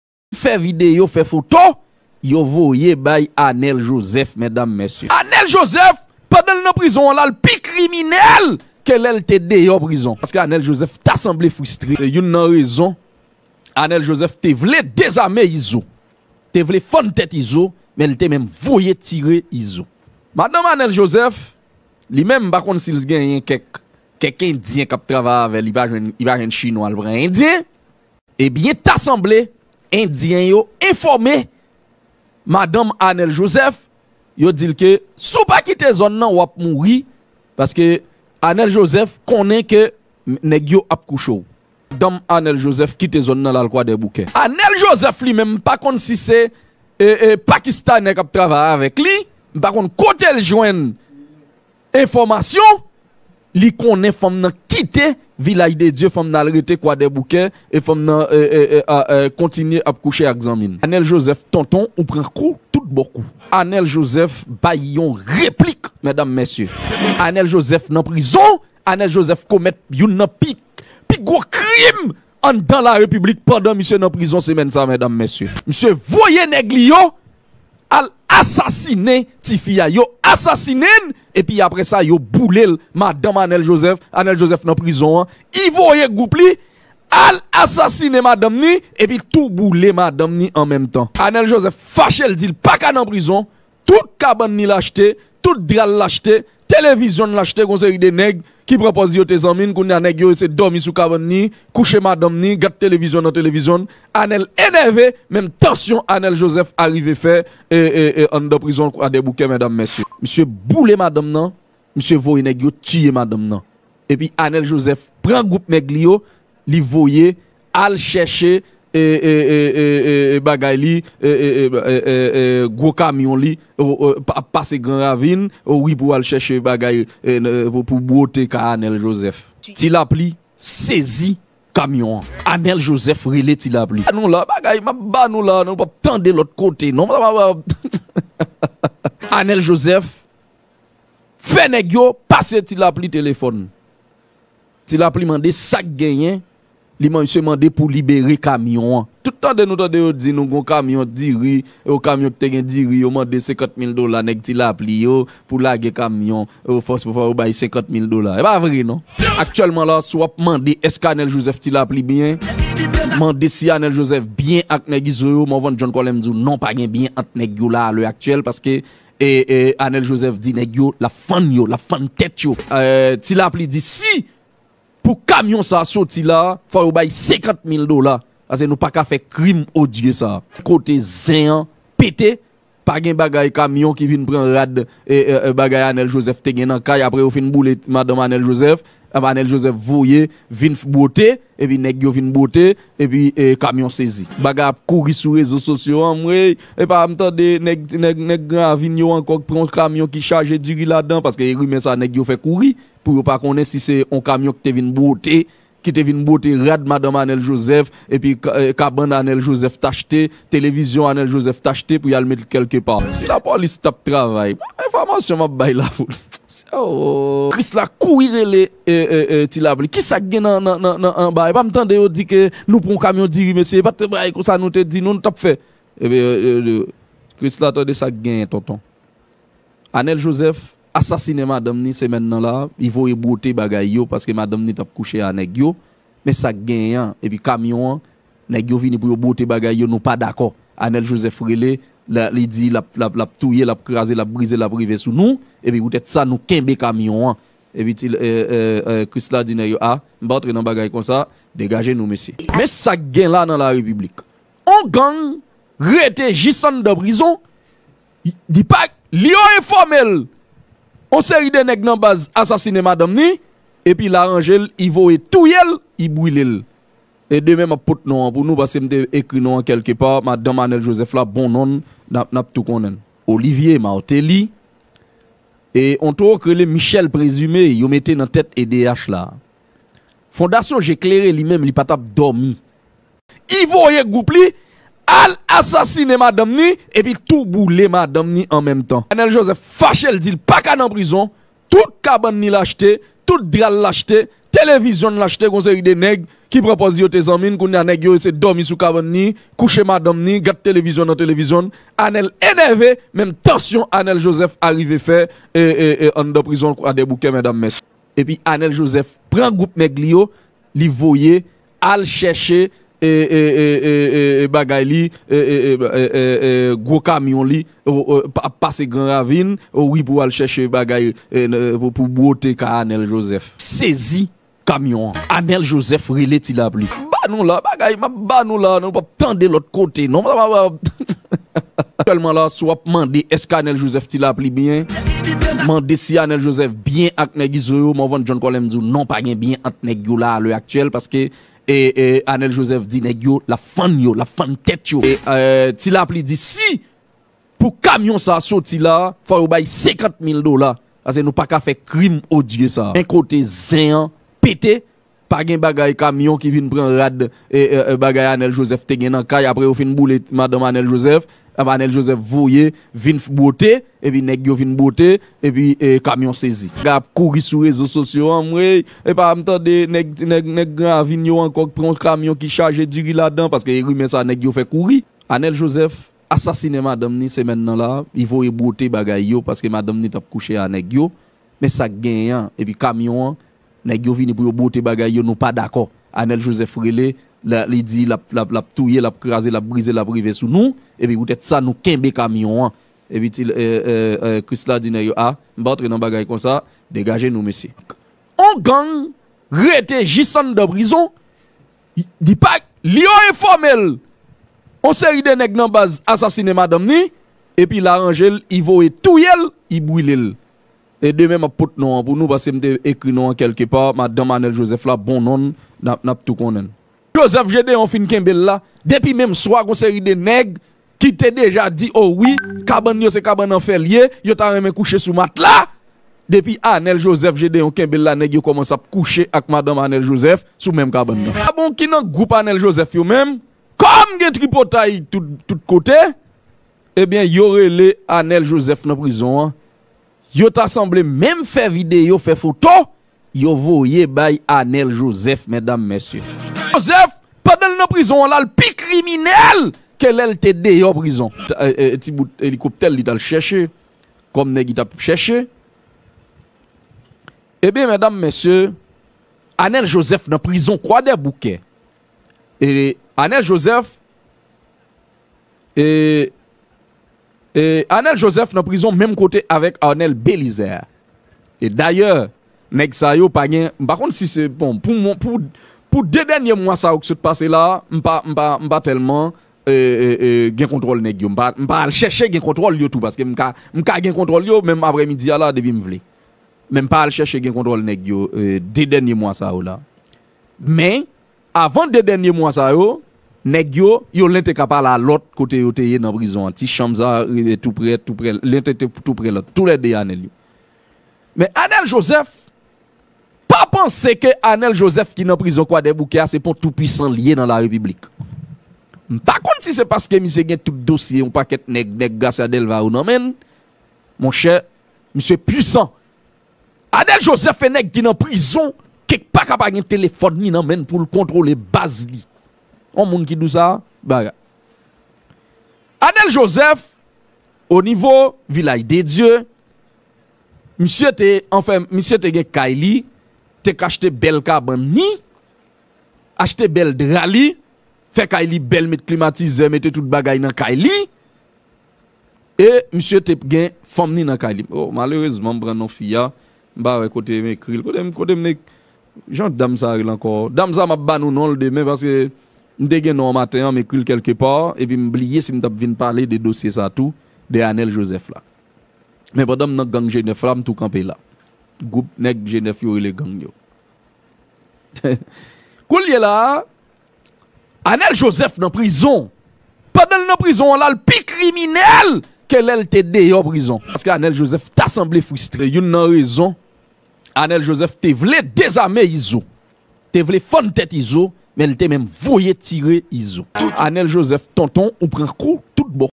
ARNEL VOYE TIYE MADANM LI LI APRANN IZO TAP KOUCHELGRO , ZEN BGY SA SE NAN FEYTON M KONN WE YO
MESI POU VIZIT OU SOU CHANEL LA. SIW SE NOUVO OU RENMEN TRAVAIL MAP FE